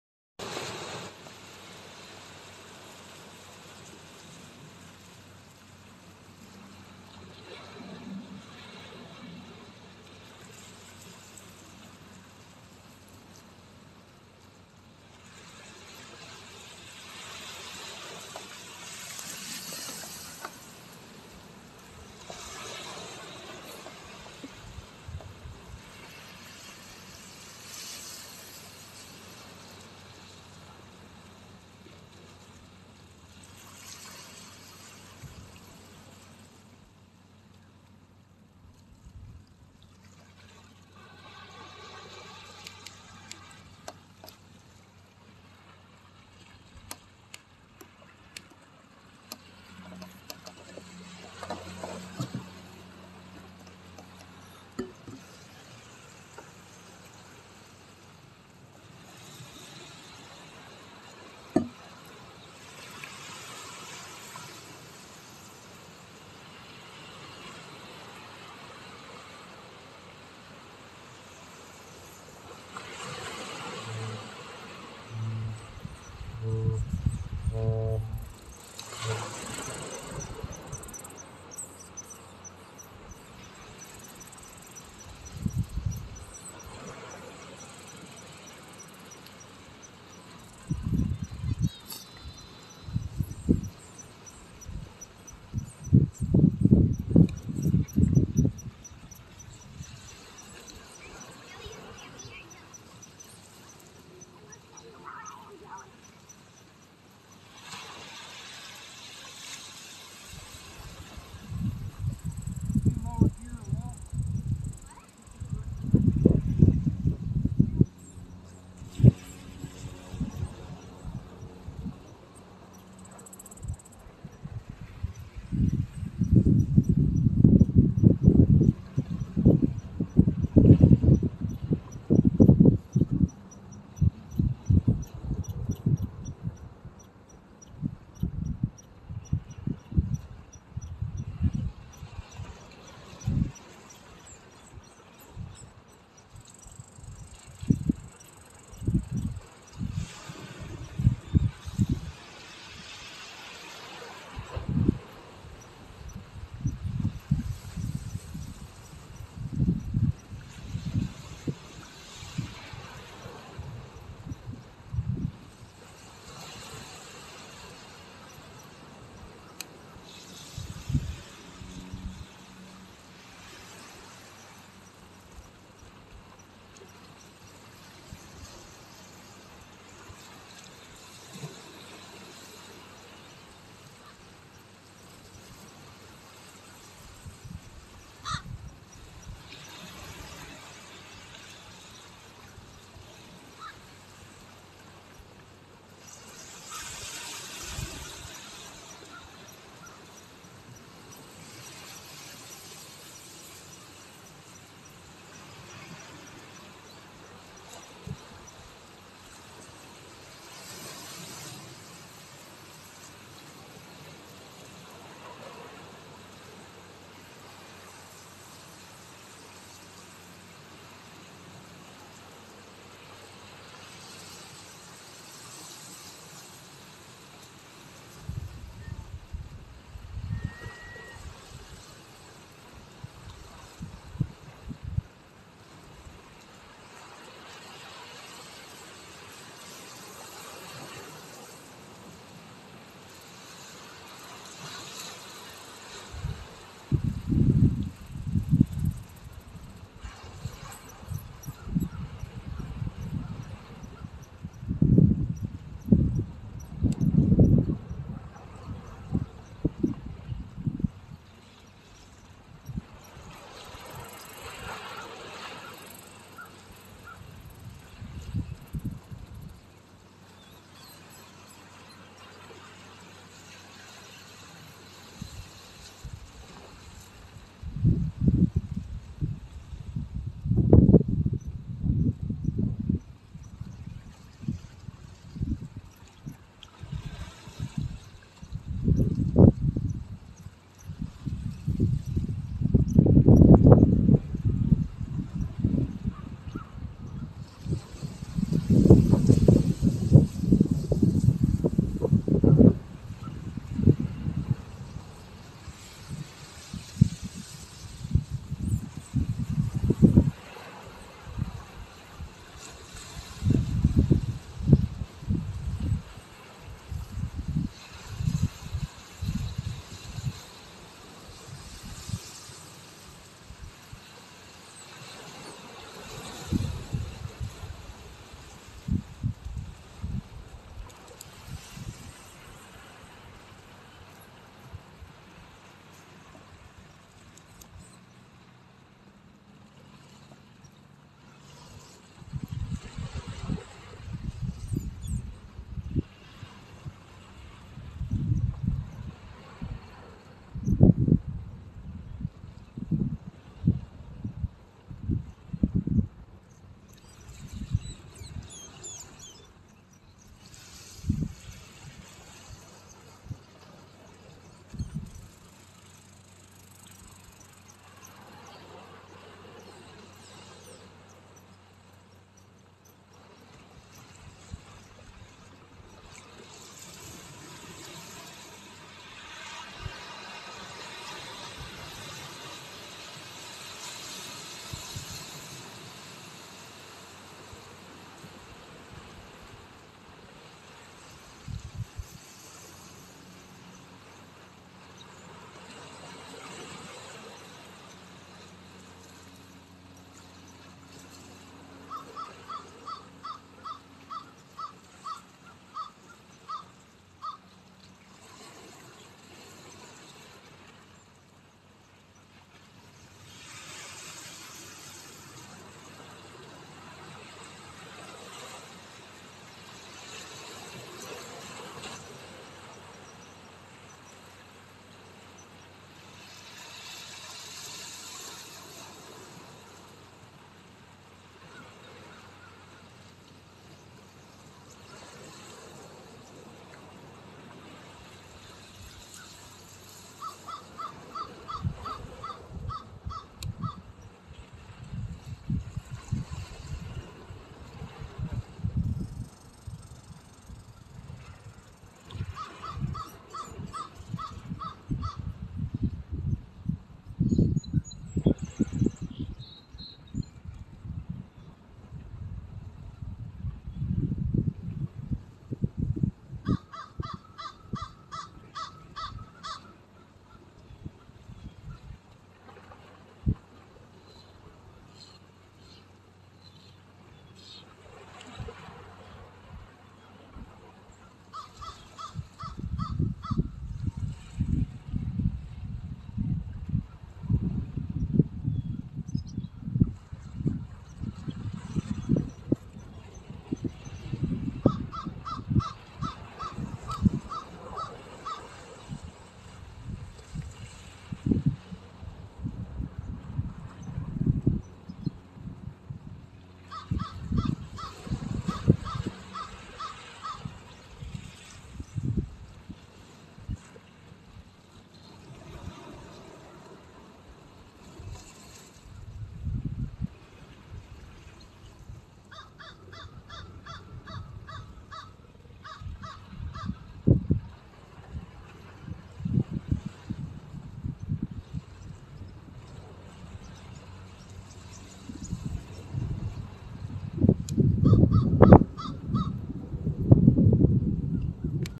Sounds of nature.
Had a beauty of a spot for brunch today along the shore of Point Pleasant Park in Halifax .